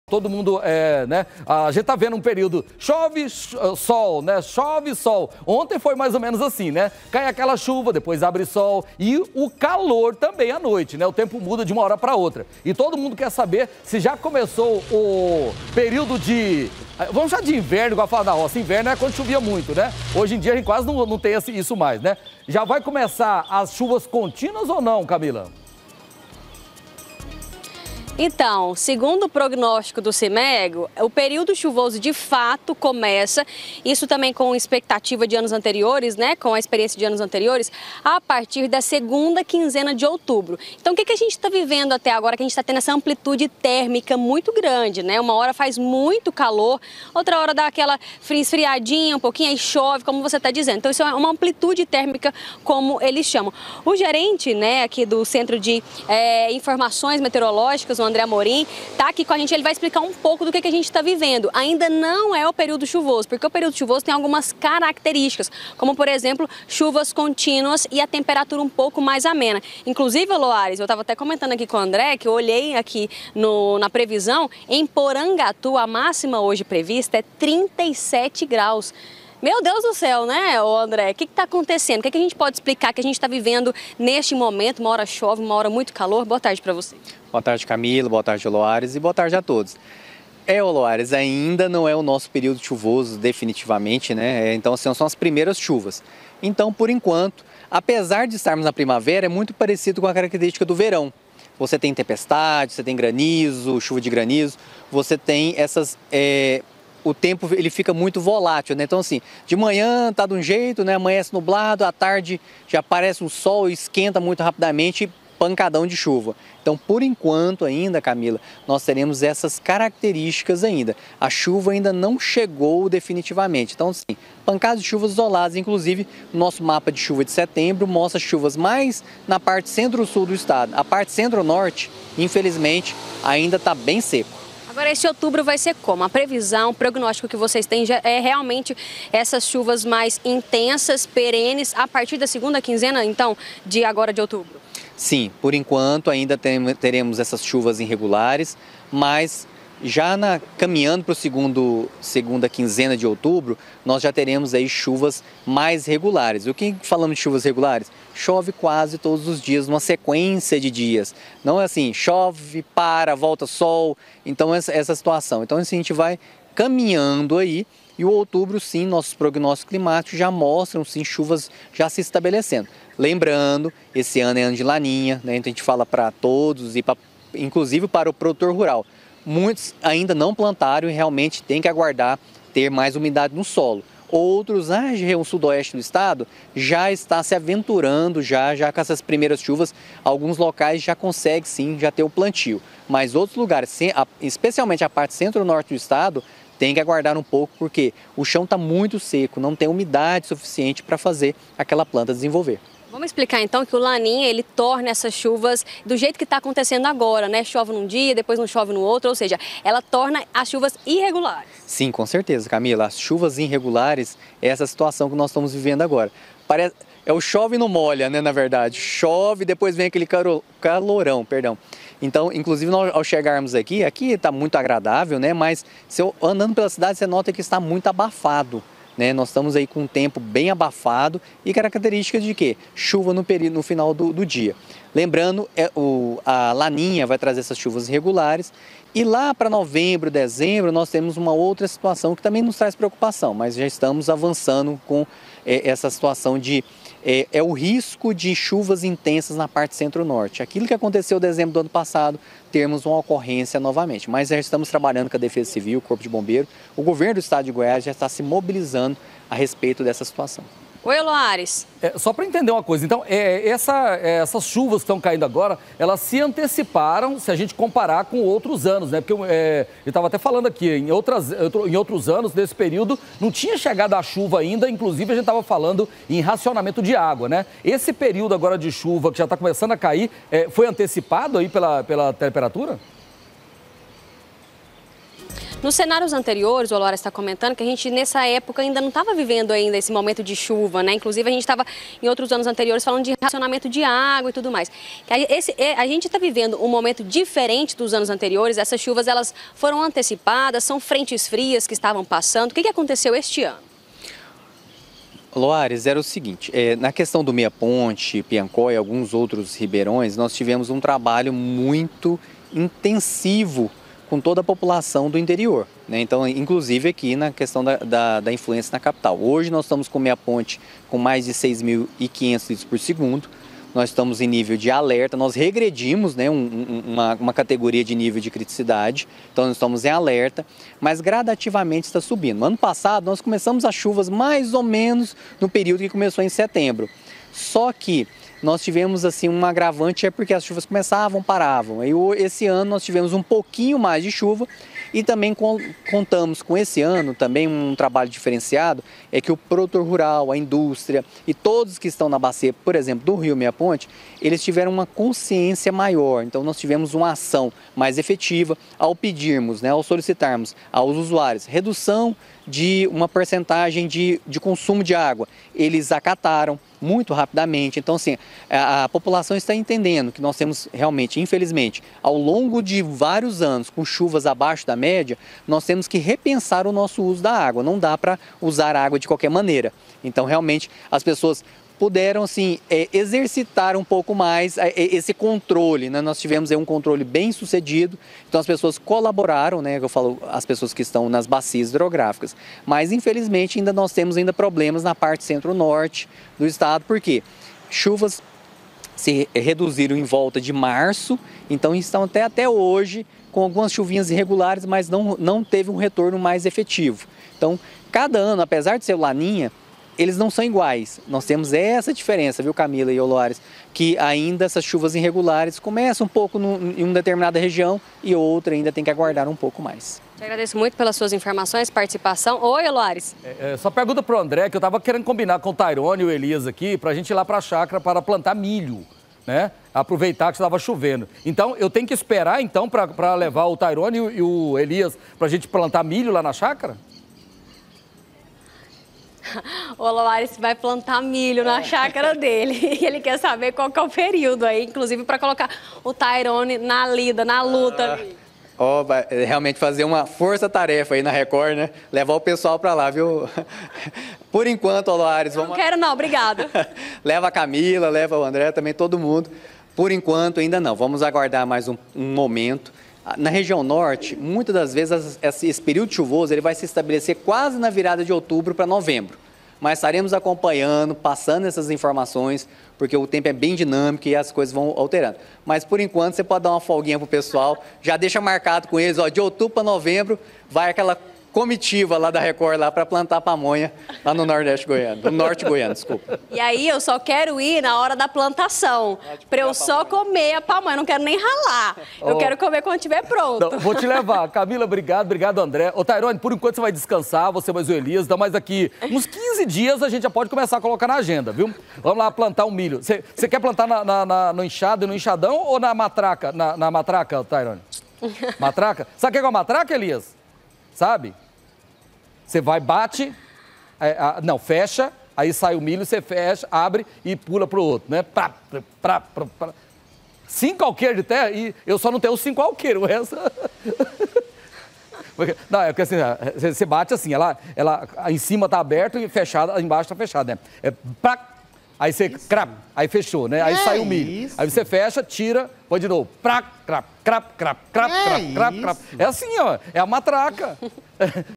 TEMPO INSTÁVEL: PREVISÃO PARA OS PRÓXIMOS DIAS
ORA CHUVA, ORA CALOR E SOL DE RACHAR. O TEMPO MUDA DE UMA HORA PRA OUTRA. E TODO MUNDO QUER SABER SE COMEÇOU A CHOVER PRA VALER. COMO DEVE FICAR O TEMPO NESSES PRÓXIMOS DIAS?